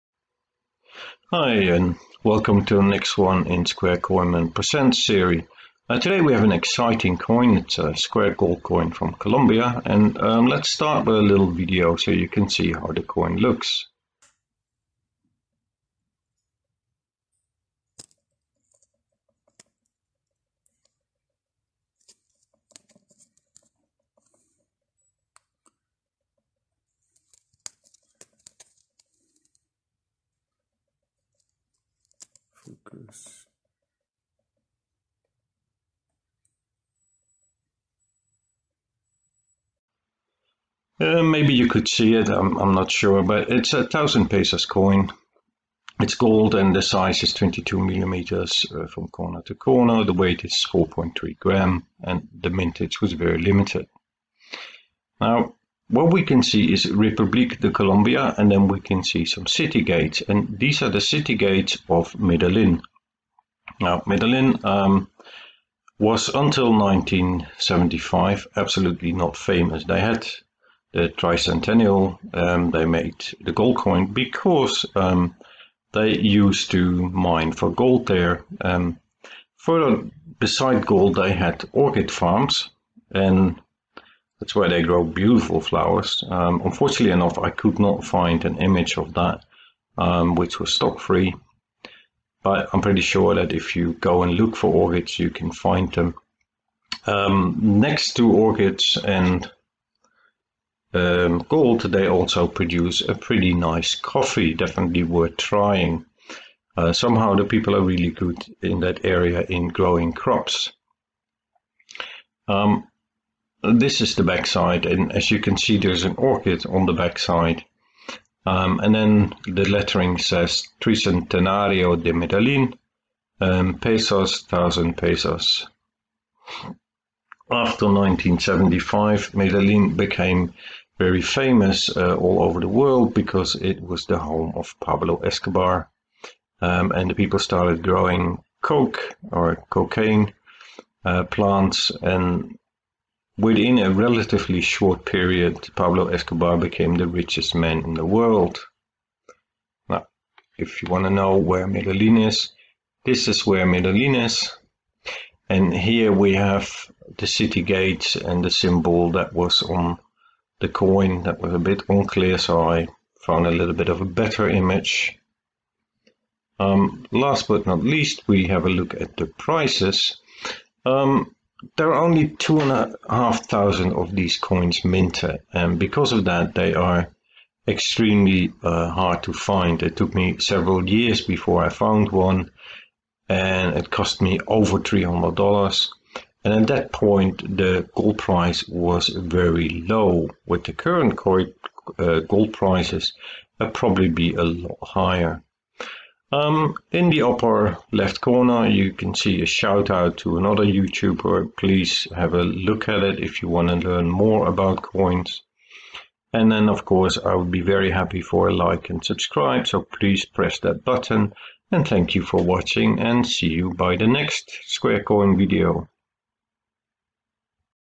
Square coin series No 6 Square gold coin of Colombia
colombiagold squarecoin 1000pesos The aim of these vidoes is to educate and share the joy of coin collecting. In this video we will look at the 1000 pesos square gold coin from Colombia a rare coin . Today there will be a shoud out to MY Coins another coin collector on youtube . That has a lot of coins to look at . Enjoy the video.